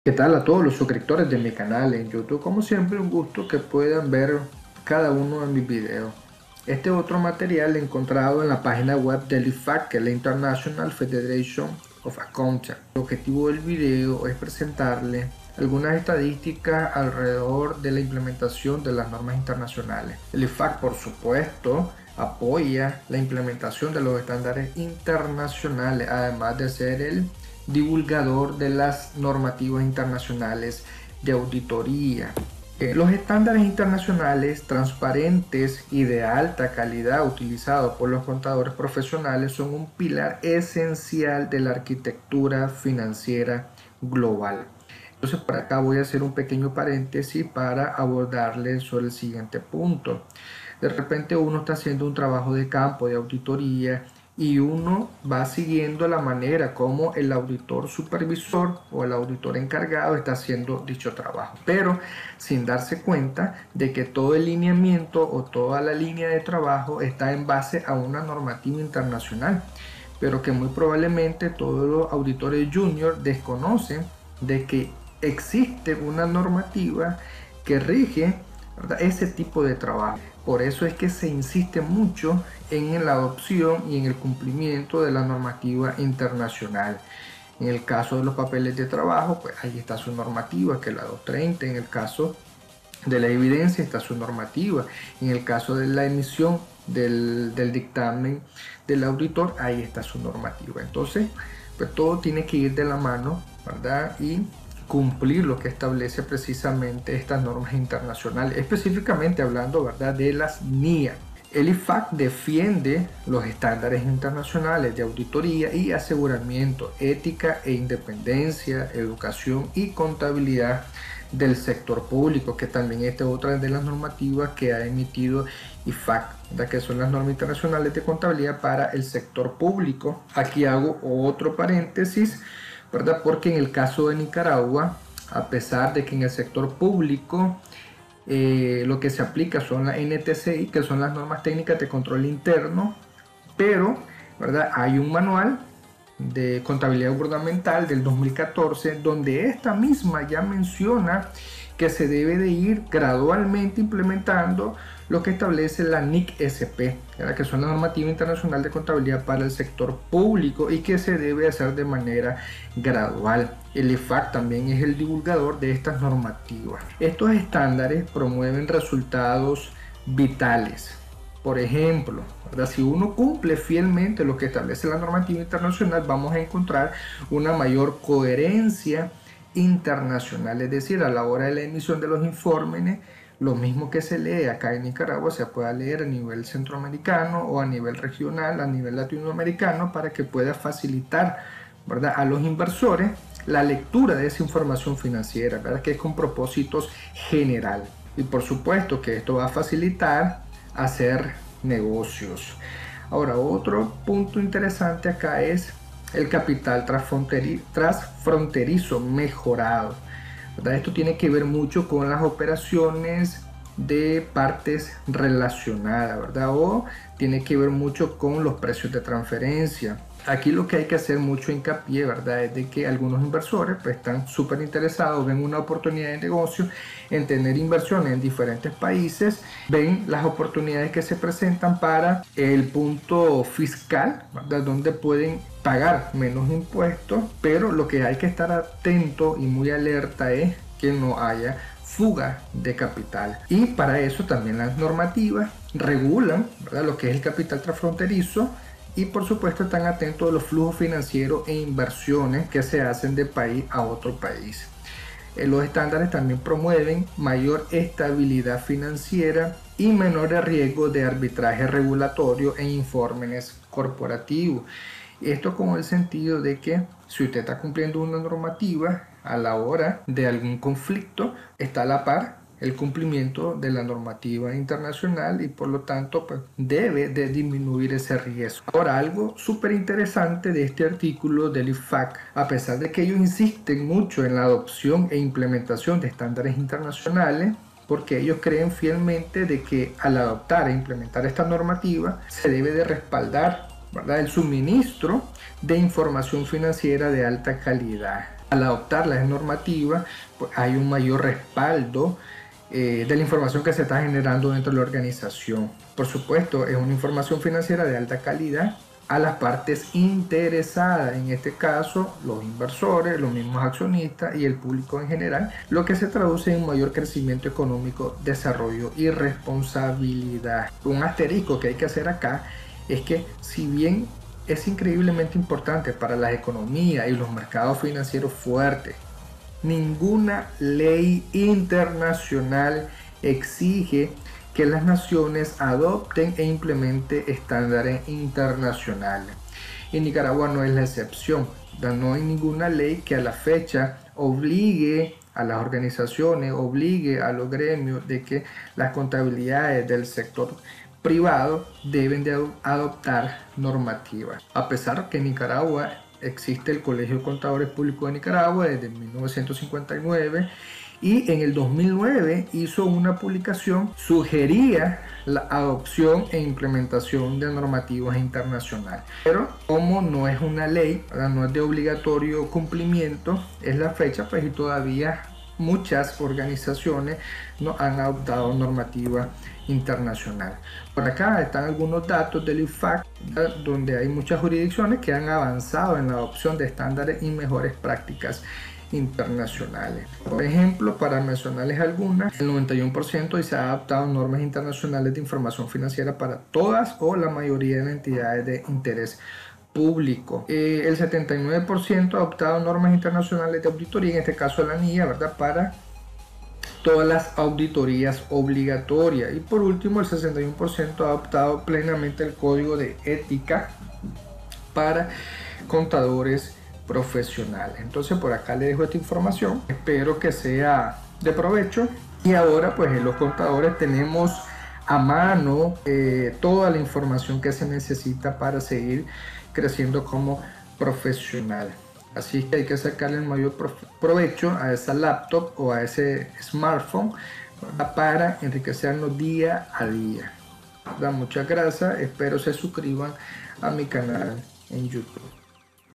APOYO A LOS ESTANDARES INTERNACIONALES-IFAC
Artículo publicado en la página Web del IFAC(Internacional Federation of Accountants) convertido a video por el Prof.Marcio Toruño Balladares